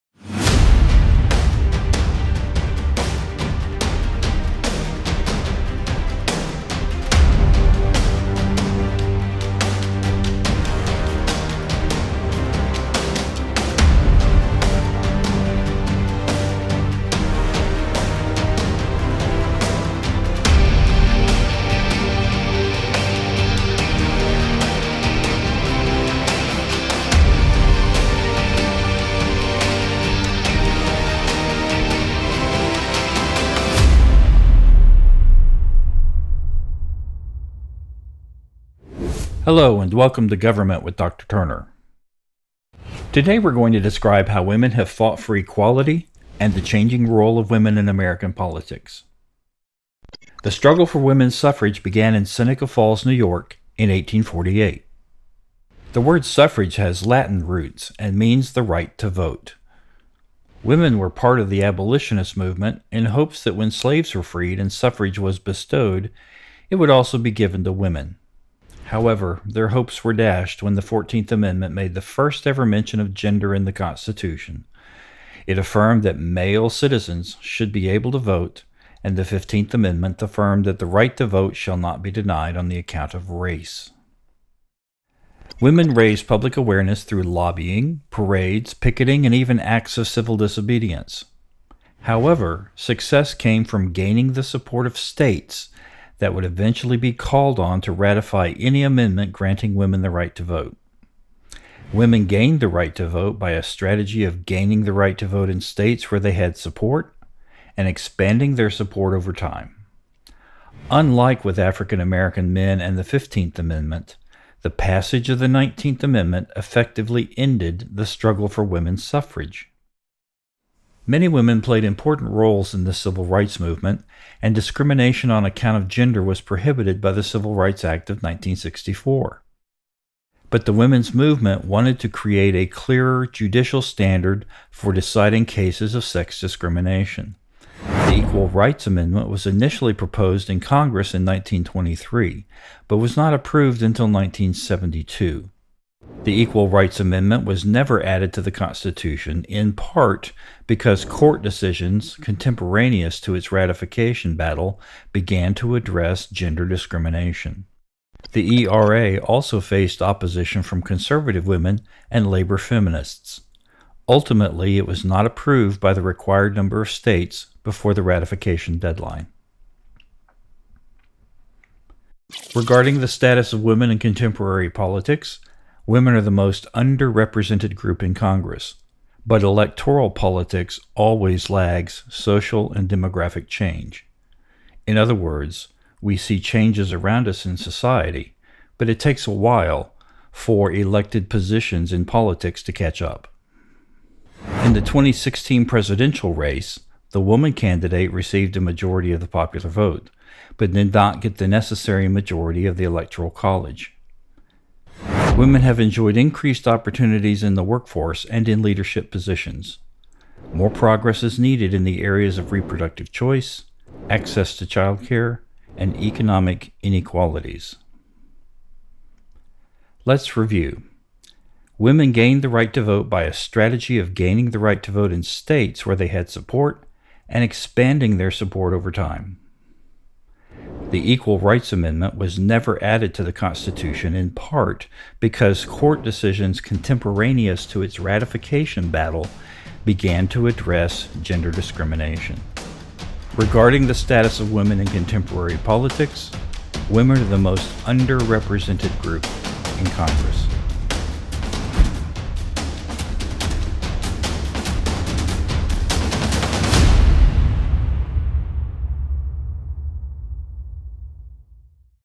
WATCH Lecture 5.4 Women's Fight for Equality
How women have fought for equality and the changing role of women in American politics.